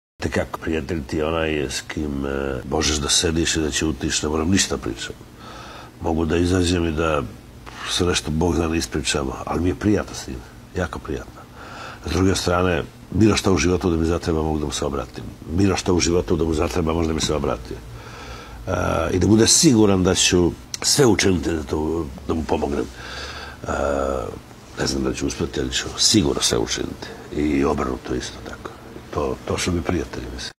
Mima Karadzić: O istinskom prijateljstvu
Milutin Mima Karadžić, jedan od naših najpoznatijih glumaca, otvoreno o iskrenom prijateljstvu.